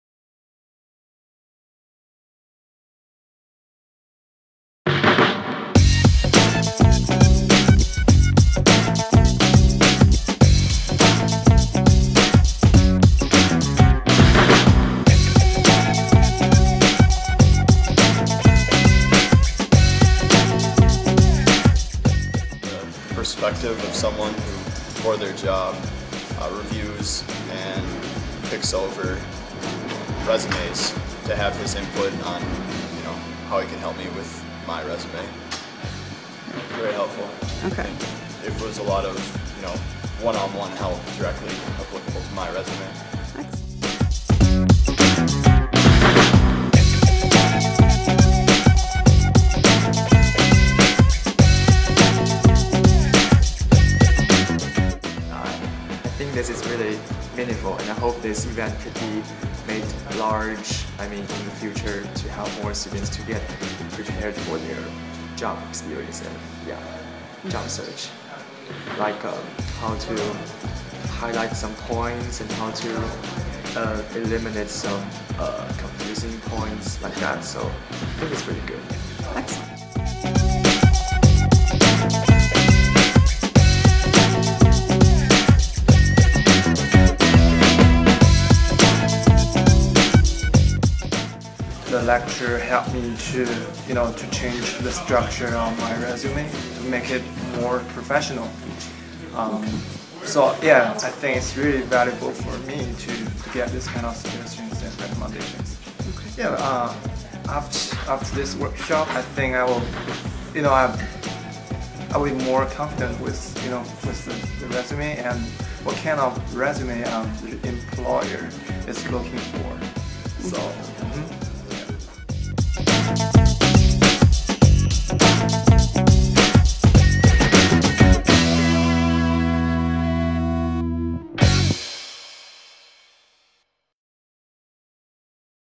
Resume Workshop Sponsored by Theta Tau and Jody Peterson
Here is a quick summary of the Resume Workshop offered by Theta Tau and Jody Peterson.